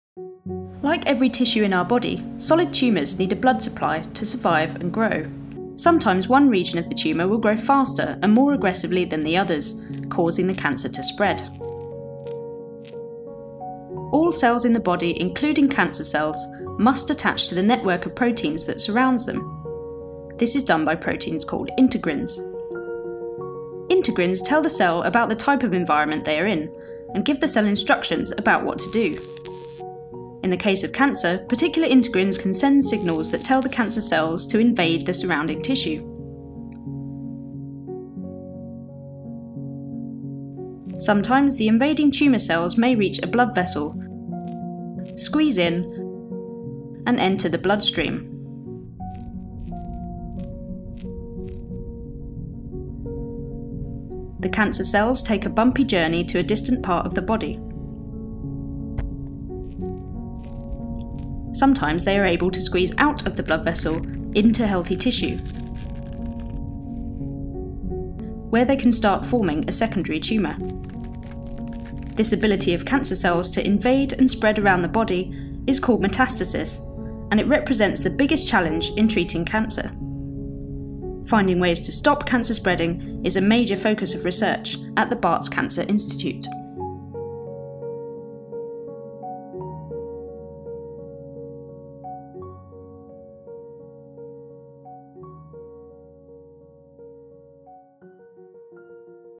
BCI Tumour Biology: integrins and metastasis
Integrins are cell surface adhesion (sticking) proteins that allow cells to interact with the environment.
In cancer these proteins can "misbehave" and this can contribute to the development of cancer, and to cancer spreading (metastasis)